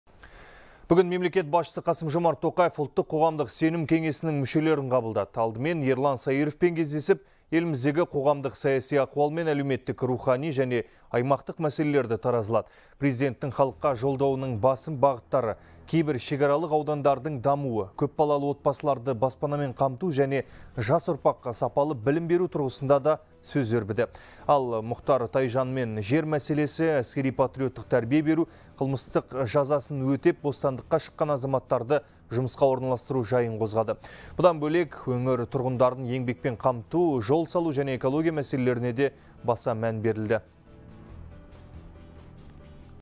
Мемлекет басшысы азаматтарды баспанамен және жұмыспен қамту мәселелерін талқылады
Қасым-Жомарт Тоқаев Ұлттық қоғамдық сенім кеңесінің мүшелері Ерлан Саиров пен Мұхтар Тайжанды қабылдады.